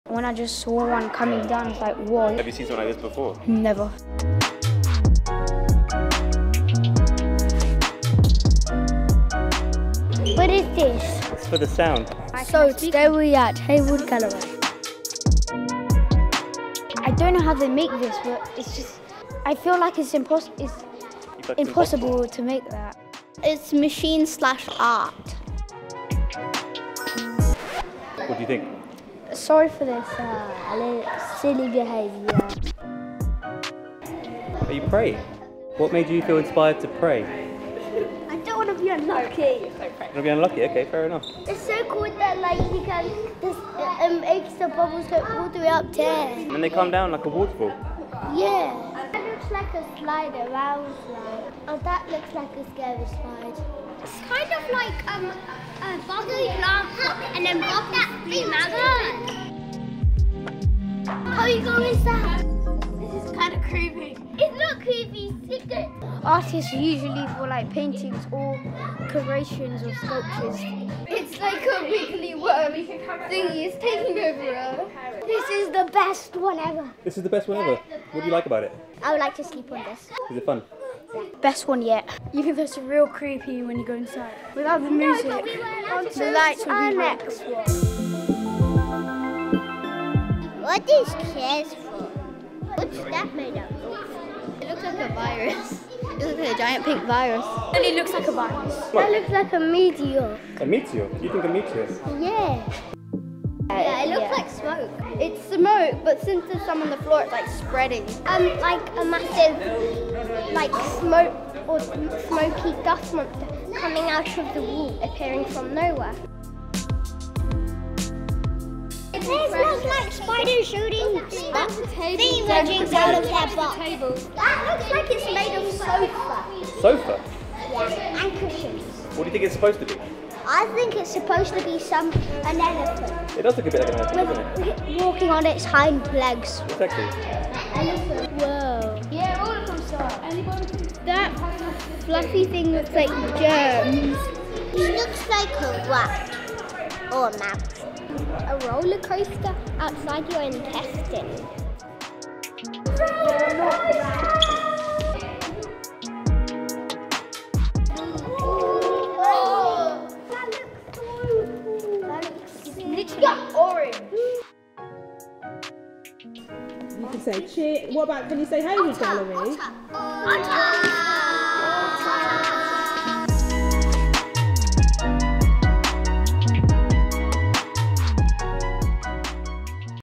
Kids React to Art: When Forms Come Alive - "That looks like it's made of sofa!"
See what a group of kids made of the artworks of When Forms Come Alive, when we gave them free rein in the Hayward Gallery exhibition.

We’re used to seeing people move around our galleries slowly and pensively as they take in artists’ work. But everyone has their own individual interaction with the art they see. So how would kids interact with the sculptures when allowed to be kids? We chose to find out, by capturing their reactions to the undulating, cascading, fluid sculptures of When Forms Come Alive in this video.

Featuring works by 21 international artists When Forms Come Alive spans 60 years of contemporary sculpture. Each work within the exhibition highlights the ways in which artists draw on and capture the experiences of movement and flux.

Among the works the children are reacting to are Shylight (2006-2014) by DRIFT, Bouquet Final (2012) by Michel Blazy, Pumping (2019) by Eva Fàbregas, Epiphanie on Stühlen (Epiphany on Chairs) (2011) by Franz West, Dream – Spontaneous – Combustion (2008) by Olaf Brzeski, Globules (2023) and Third Instar (2023) both by Matthew Ronay and A Subsequent Offering (2017) by EJ Hill.

When Forms Come Alive is at Hayward Gallery until 6 May 2024.